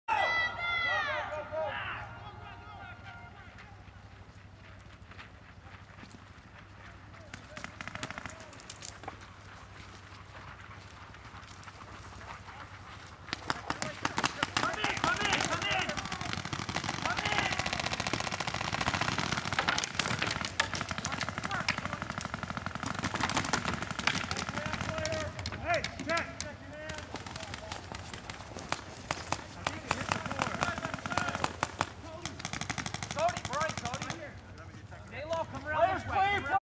XCGI Event: Field Location - Village East 2
Field Location: Village East

This event was held at A Pirates Paradise.
This is a Randy Couture charity event.